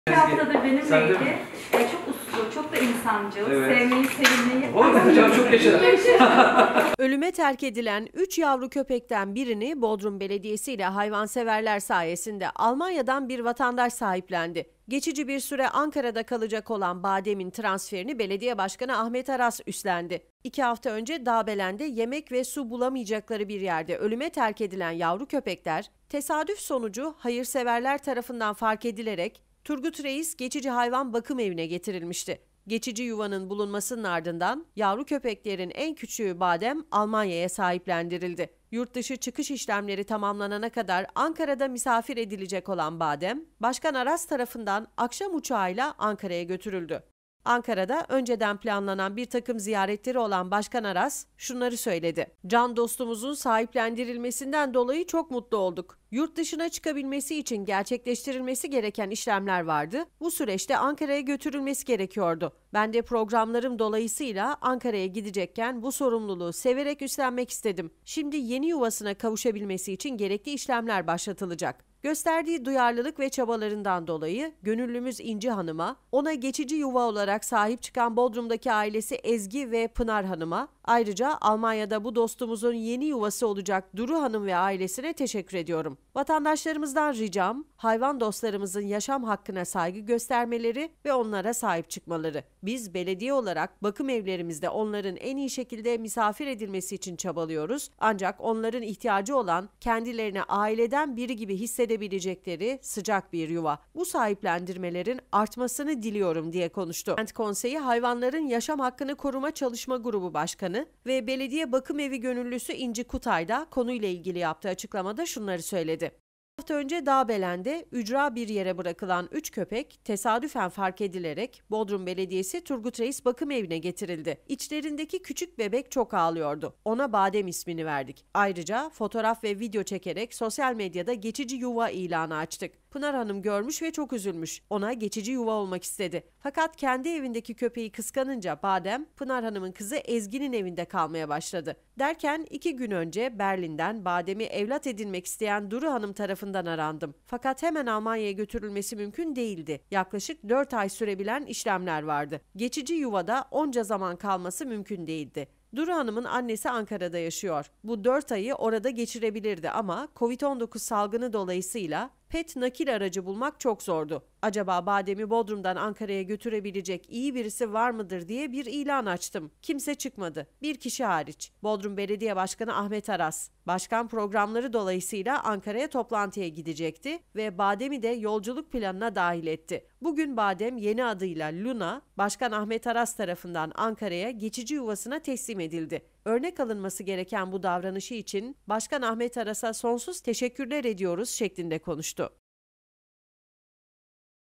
BİR DOKUNUŞLA HAYATI DEĞİŞTİ
Ölüme terk edilen üç yavru köpekten birini, Bodrum Belediyesi ile Hayvanseverler sayesinde Almanya'dan bir vatandaş sahiplendi. Geçici bir süre Ankara'da kalacak olan "Badem"in transferini Belediye Başkanı Ahmet Aras üstlendi.
26 Ocak 2021 14:230
Bir Dokunuşla Hayatı Değişti...
İki hafta önce Dağbelen’de yemek ve su bulamayacakları bir yerde ölüme terk edilen yavru köpekler, tesadüf sonucu hayvanseverler tarafından fark edilerek Turgutreis Geçici Hayvan Bakımevi’ne getirilmişti. Geçici yuvanın bulunmasının ardından, yavru köpeklerin en küçüğü Badem, Almanya’ya sahiplendirildi. Yurtdışı çıkış işlemleri tamamlanana kadar Ankara’da misafir edilecek olan Badem, Başkan Aras tarafından akşam uçağıyla Ankara’ya götürüldü.

BAŞKAN ARAS:"SAHİPLENMELERİN ARTMASI DİLİYORUM"

Ankara’da önceden planlanan birtakım ziyaretleri olan Başkan Aras, "Can dostumuzun sahiplendirilmesinden dolayı çok mutlu olduk. Yurtdışına çıkabilmesi için gerçekleştirilmesi gereken işlemler vardı, bu süreçte Ankara’ya götürülmesi gerekiyordu. Ben de programlarım dolayısıyla Ankara’ya gidecekken bu sorumluluğu severek üstlenmek istedim. Şimdi yeni yuvasına kavuşabilmesi için gerekli işlemler başlatılacak. Gösterdiği duyarlılık ve çabalarından dolayı gönüllümüz İnci Hanım’a, ona geçici yuva olarak sahip çıkan Bodrum’daki ailesi Ezgi ve Pınar Hanım’a, ayrıca Almanya’da bu dostumuzun yeni yuvası olacak Duru Hanım ve ailesine teşekkür ediyorum. Vatandaşlarımızdan ricam, hayvan dostlarımızın yaşam hakkına saygı göstermeleri ve onlara sahip çıkmaları. Biz belediye olarak bakımevlerimizde onların en iyi şekilde misafir edilmesi için çabalıyoruz, ancak onların ihtiyacı olan kendilerini aileden biri gibi hissedebilecekleri sıcak bir yuva. Bu sahiplendirmelerin artmasını diliyorum" diye konuştu.

Kent Konseyi Hayvanların Yaşam Hakkını Koruma Çalışma Grubu Başkanı ve belediye bakımevi gönüllüsü İnci Kutay da konuyla ilgili yaptığı açıklamada şunları söyledi:

"İki hafta önce Dağbelen’de ücra bir yere bırakılan 3 köpek, tesadüfen fark edilerek Bodrum Belediyesi Turgutreis Bakımevi’ne getirildi. İçlerindeki küçük bebek çok ağlıyordu. Ona Badem ismini verdik, ayrıca fotoğraf ve video çekerek sosyal medyada geçici yuva ilanı açtık. Pınar Hanım görmüş ve çok üzülmüş, ona geçici yuva olmak istedi. Fakat kendi evindeki köpeği kıskanınca Badem, Pınar Hanım’ın kızı Ezgi’nin evinde kalmaya başladı. Derken iki gün önce Berlin’den Badem’i evlat edinmek isteyen Duru Hanım tarafından arandım. Fakat hemen Almanya’ya götürülmesi mümkün değildi, yaklaşık dört ay sürebilen işlemler vardı. Geçici yuvada onca zaman kalması mümkün değildi. Duru Hanım’ın annesi Ankara’da yaşıyor. Bu dört ayı orada geçirebilirdi ama Covid 19 salgını dolayısı ile Pet nakil aracı bulmak çok zordu. Acaba Badem’i Bodrum’dan Ankara’ya götürebilecek iyi birisi var mıdır diye bir ilan açtım. Kimse çıkmadı. Bir kişi hariç. Bodrum Belediye Başkanı Ahmet Aras. Başkan programları dolayısıyla Ankara’ya toplantıya gidecekti ve Badem’i de yolculuk planına dahil etti. Bugün Badem yeni adıyla Luna, Başkan Ahmet Aras tarafından Ankara’ya, geçici yuvasına teslim edildi. Örnek alınması gereken bu davranışı için Başkan Ahmet Aras’a sonsuz teşekkürler ediyoruz" şeklinde konuştu.

Badem, gerekli işlemlerin tamamlanmasıyla Almanya’daki yeni yuvasına kavuşacak.

Öte yandan, terk edilen diğer iki yavru ise diğer canlar gibi Bodrum Belediyesi Geçici Hayvan Bakımevi ve Rehabilitasyon Merkezinde sahiplenilmeyi bekliyorlar.